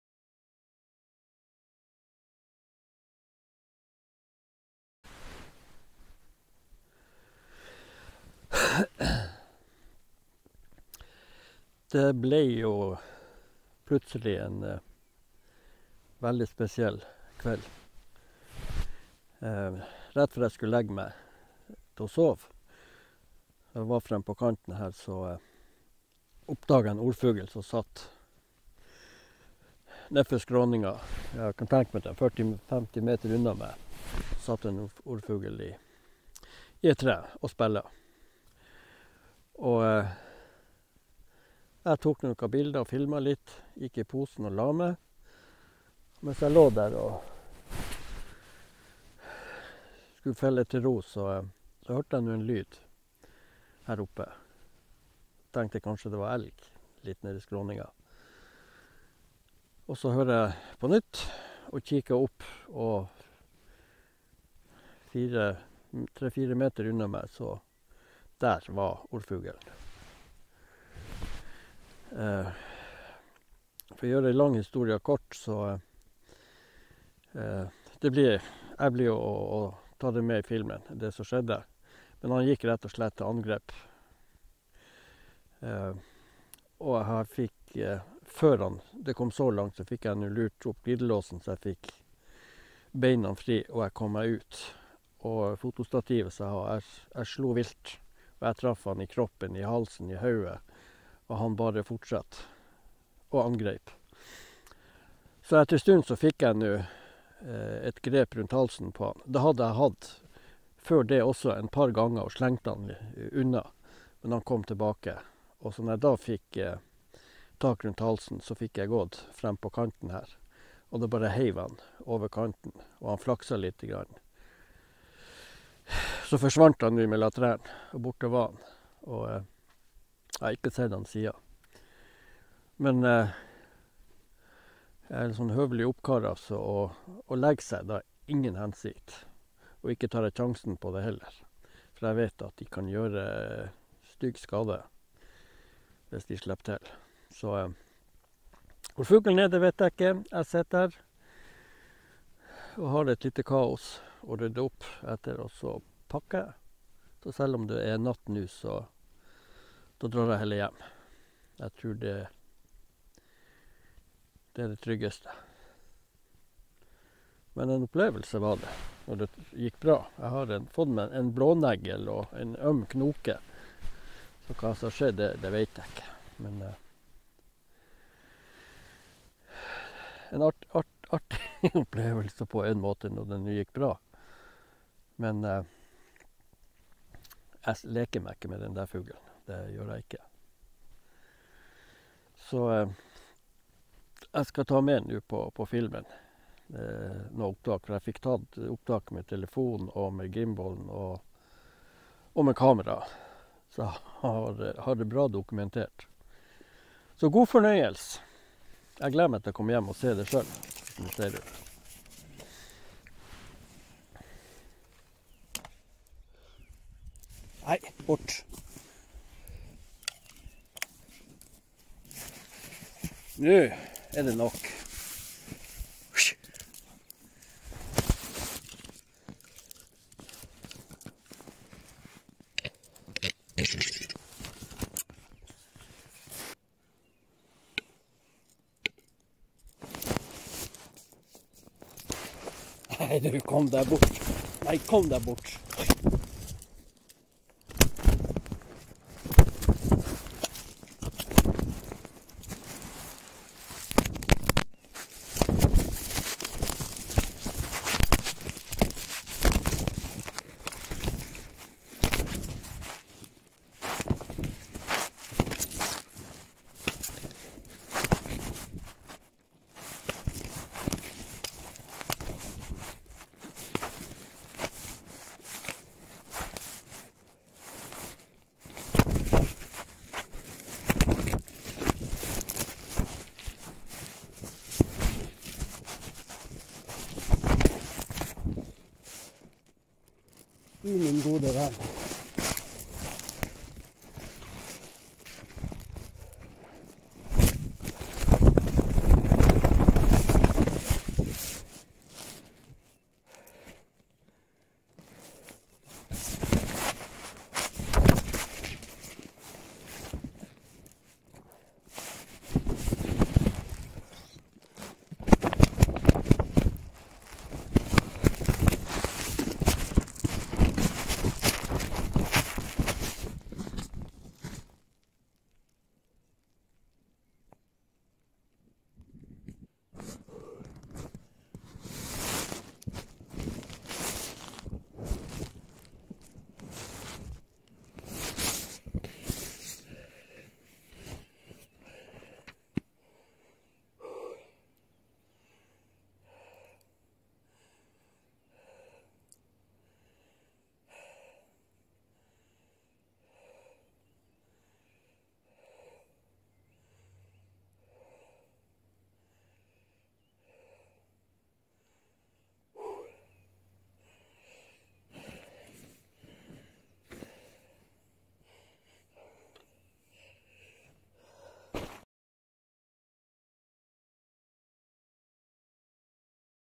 Spillende tiur i angrep
Det som skulle bli ei behagelig overnatting i hengekøye ble til overnatting under åpen himmel på mose og røsslyng. Det vil si, det ble ikke overnatting, selv om jeg prøvde såvidt. Takket være et illsint fjærkre. Rettere sagt en tiur.
Se filmen, den er ganske kul, om jeg skal si det selv.

DET KOMMER FILM FRA HELE TUREN, LEGGES UT OFFENTLIG 25. MAI.